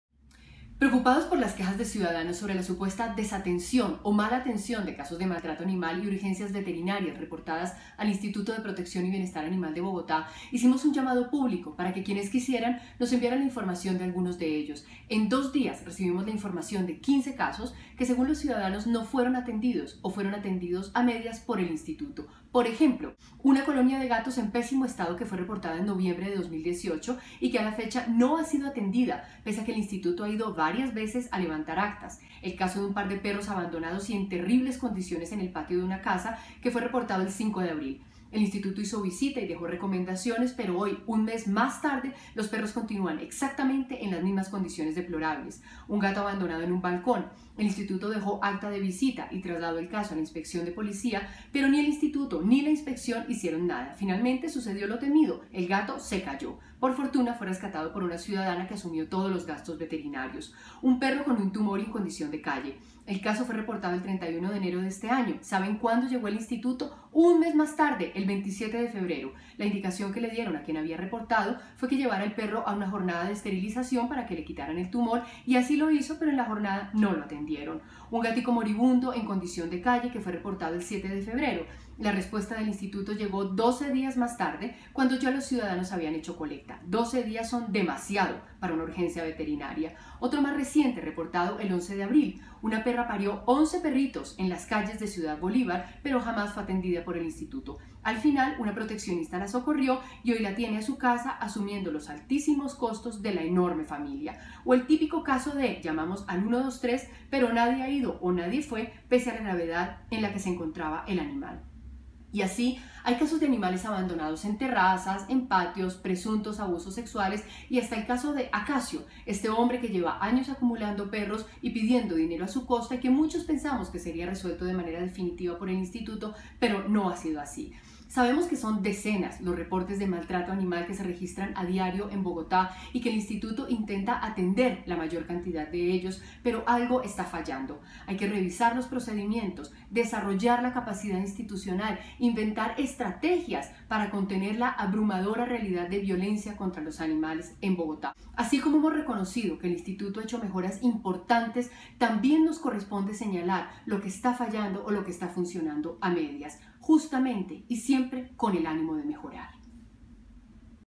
Denuncia. Instituto de Protección y Bienestar Animal de Bogotá
Caso no atendidos o mal atendidos por el Instituto de Protección y Bienestar Animal de Bogotá, Colombia (IDPYBA): otro frente por mejorar. Recogemos quejas ciudadanas. Mayo 10 de 2019.